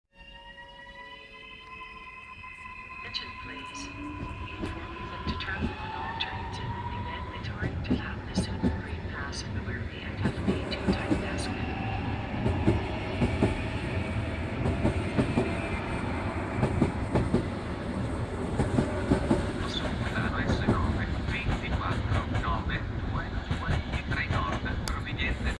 ETR421 017+026 Trenord - Milano Greco - 25/03/2022
S8 24823 Lecco-Milano Porta Garibaldi effettuato con gli ETR421 017 e 026 Trenord.